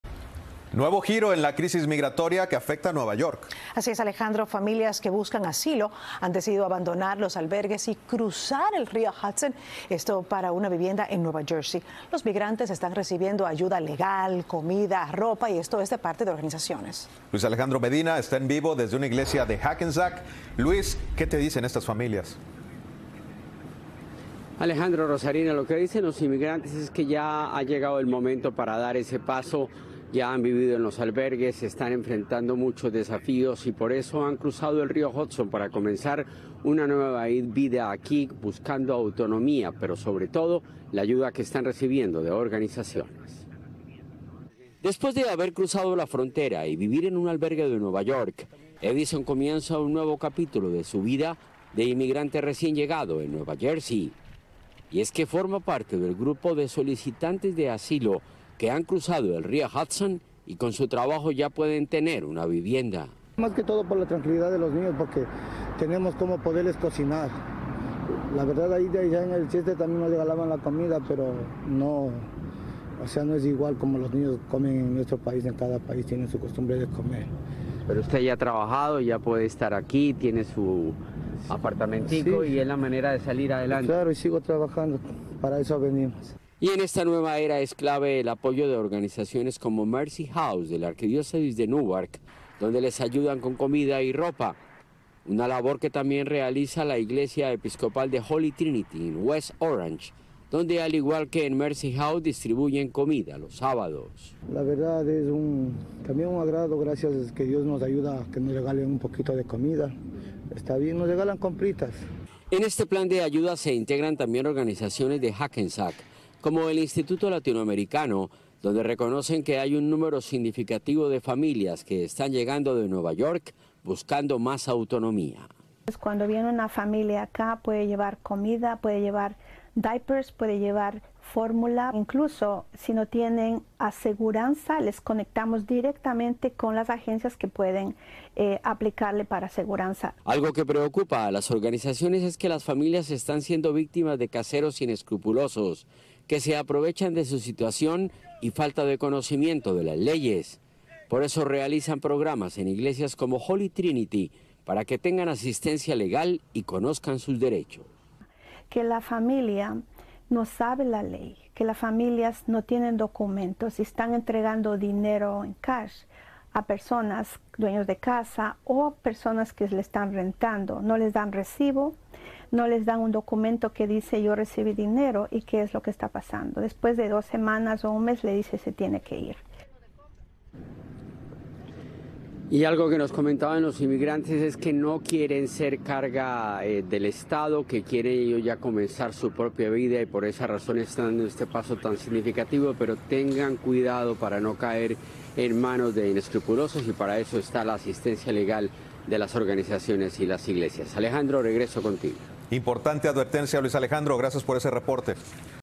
Organizaciones de Nueva Jersey ofrecen ayuda para migrantes
Luis Alejandro Medina nos amplía desde Hackensack, Nueva Jersey.